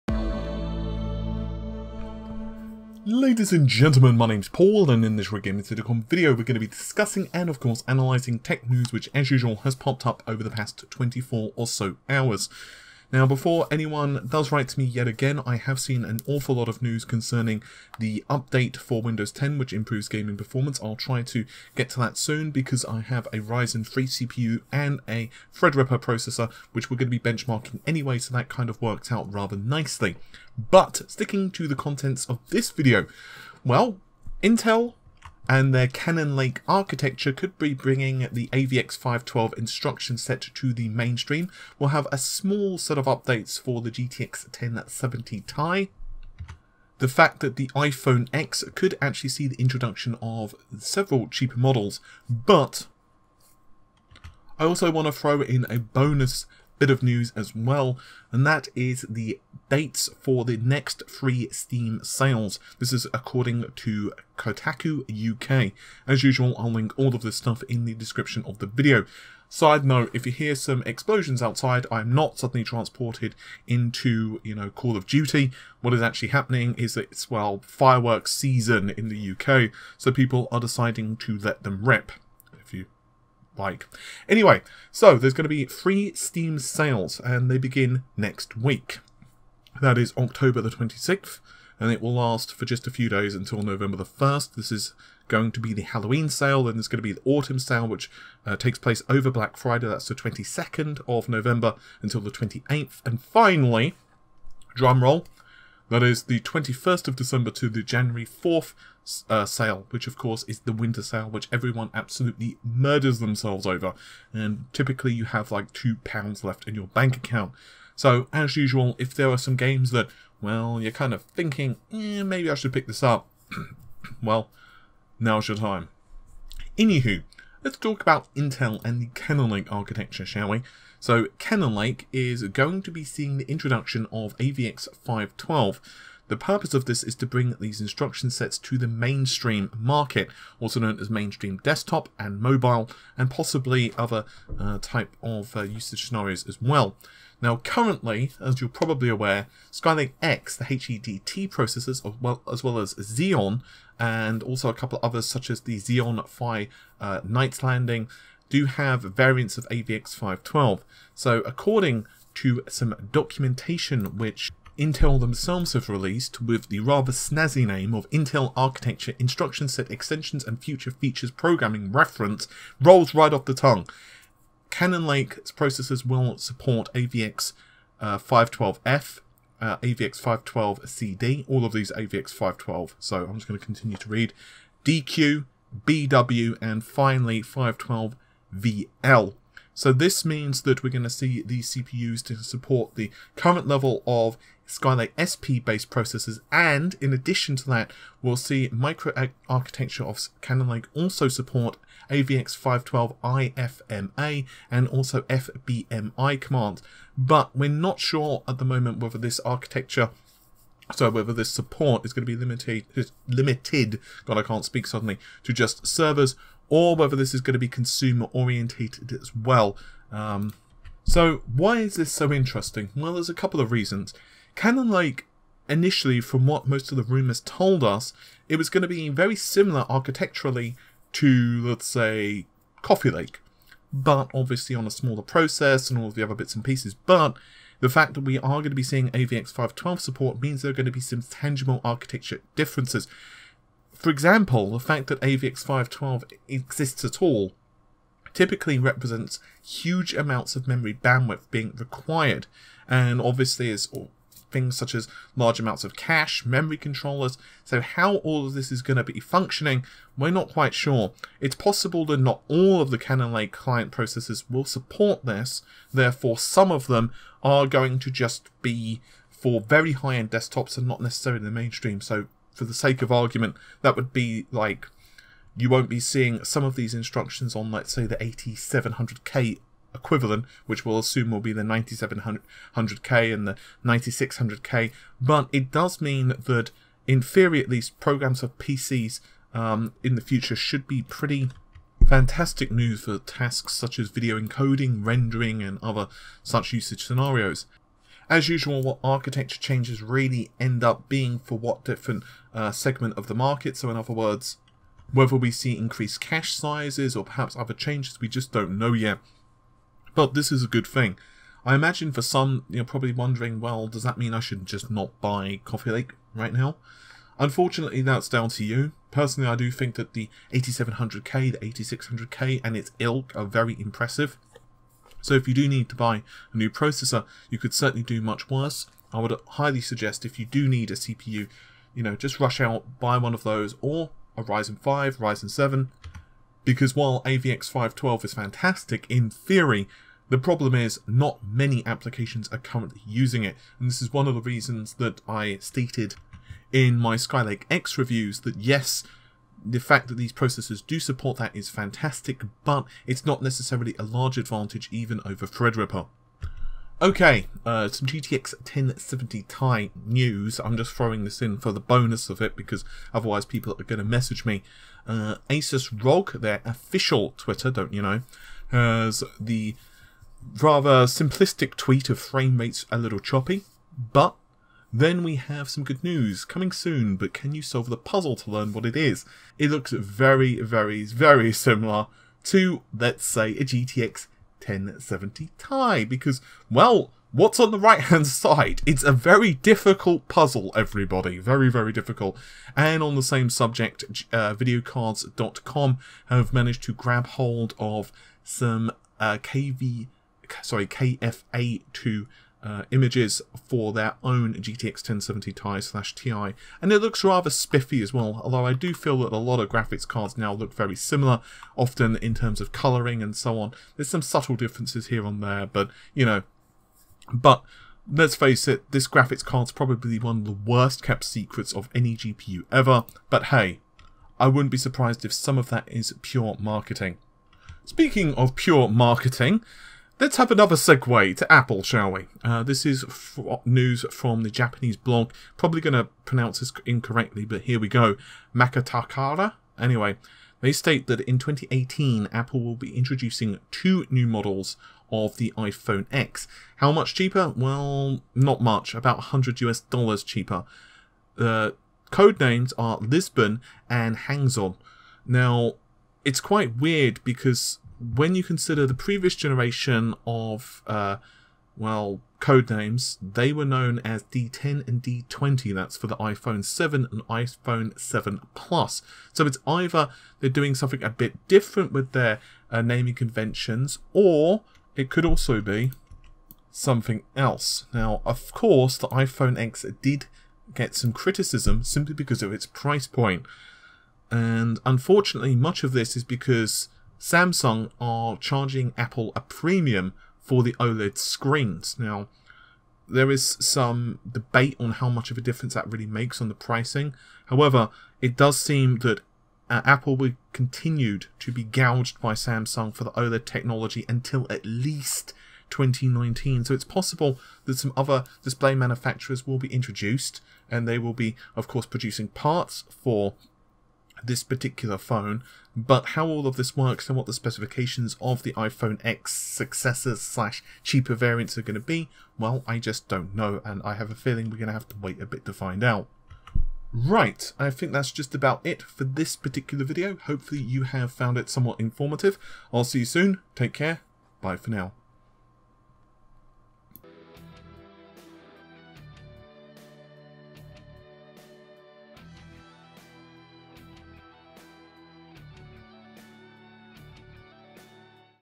Cannon Lake Architecture Gets Major Update | More 1070 Ti Leaks | iPhone X Cheaper Models
Intel's Cannon Lake on the mainstream might well be receiving an update which would allow them to run AVX512 instruction sets, along with several other smaller changes. This might mean there's some deeper underlying changes to the cannon lake architecture which hasn't been published yet other than a simple process shrink. AVX512F, AVX512CD, AVX512DQ, AVX512BW, and AVX512VL instructions, and will support AVX512_IFMA and AVX512_VBMI commands,

GeForce gtx 1070 ti graphics card does seem to exist (shocking) as it has been leaked by KFA2 and ASUS Rog have hinted it.

lastly, the Iphone x will have cheaper models released next year.